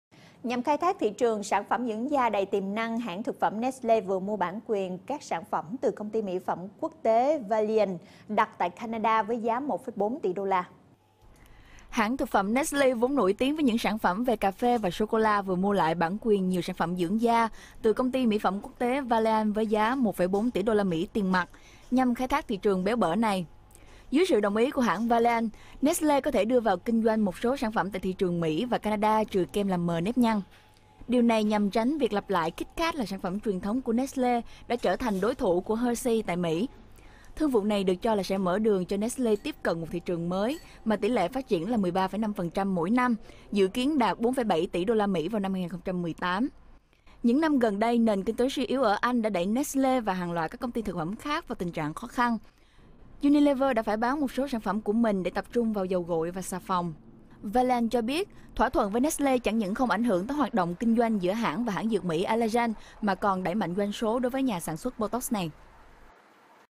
Nestle lấn sân sang lĩnh vực mỹ phẩm
Nhằm khai thác thị trường sản phẩm dưỡng da đầy tiềm năng, hãng thực phẩm Nestle vừa mua bản quyền các sản phẩm từ công ty mỹ phẩm quốc tế Valeant đặt tại Canada với giá 1,4 tỷ USD.